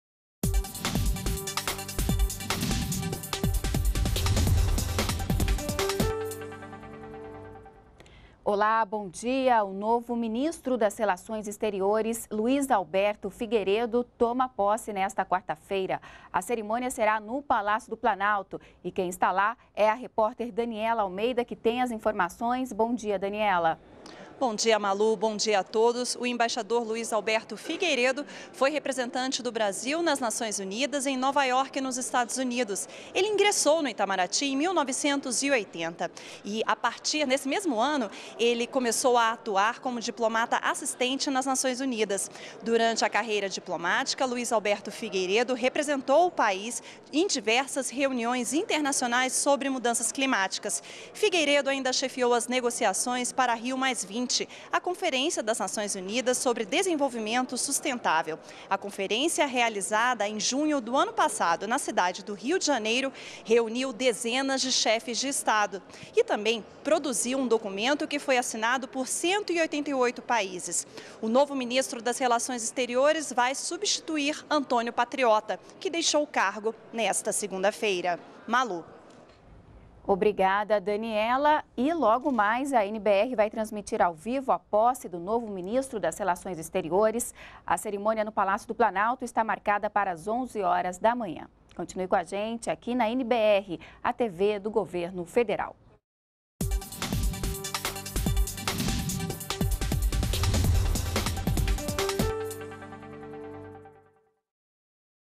Novo ministro das Relações Exteriores toma posse em Brasília
ATIVIDADES DA PRESIDENTA - 28.08.13: Nesta quarta-feira (28), o novo ministro das Relações Exteriores, Luiz Alberto Figueiredo, toma posse em Brasília. Antes de assumir o cargo, o embaixador foi representante do Brasil na Organização das Nações Unidas (ONU) em Nova Iorque.  Durante a carreira diplomática, Figueiredo representou o Brasil em diversas reuniões internacionais sobre mudanças climáticas e chefiou as negociações para Rio+20 realizada em junho de 2012. O novo ministro vai substituir Antonio Patriota.